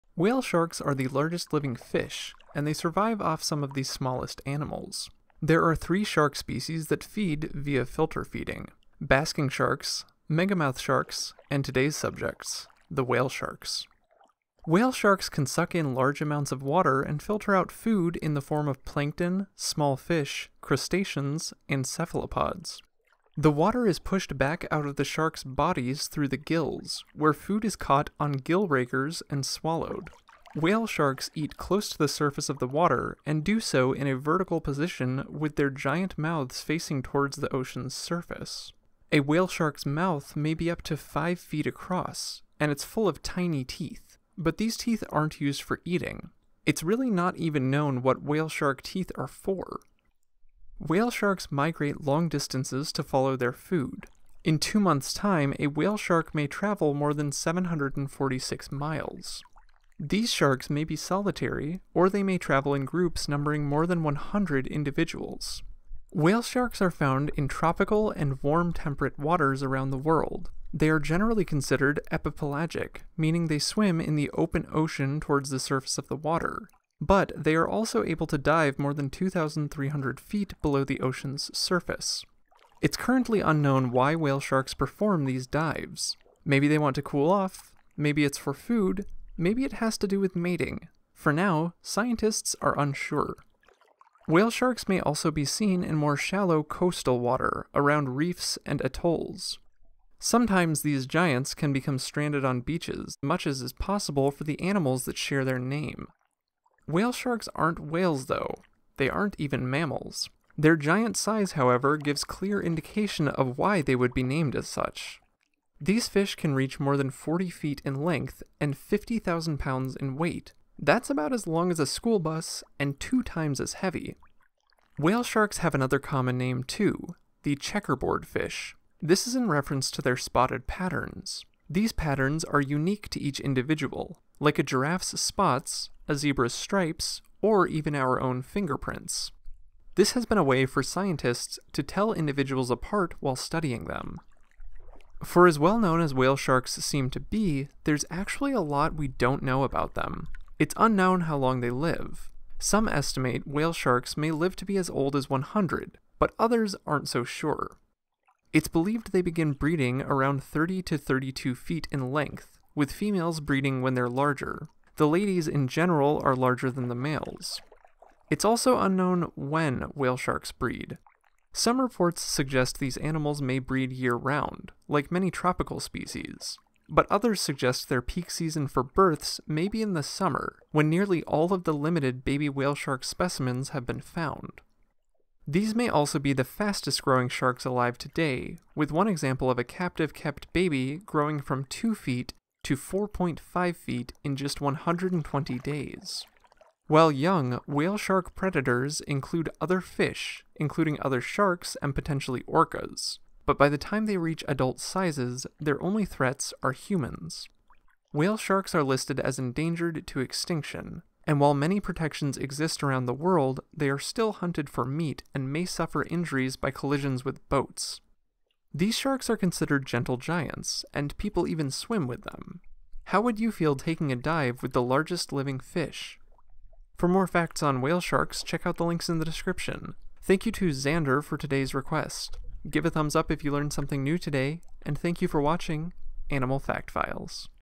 Whale Shark facts: not actually a whale | Animal Fact Files
Whale sharks are the largest living fish in our ocean. While the ocean sunfish might take the spot as the largest living *bony* fish, whale sharks have skeletons made of cartilage! Whale sharks may also be called checkerboard sharks in reference to their unique spots. These fish are considered gentle giants and may also be surrounded by thousands of smaller fish. Don't be fooled, though, if whale sharks get too hungry, they will happily eat these fish using the whale sharks for protection. Unfortunately, whale sharks are endangered and their slow moving nature makes them vulnerable to anthropogenic threats including hunting and boat collisions.

Scientific Name: Rhincodon typus
Range: tropical and temperate water worldwide
Size: 40+ feet (12+m); 50,000lbs (22,680kg)
Diet: plankton, crustaceans, cephalopods, small fish
Lifespan: unknown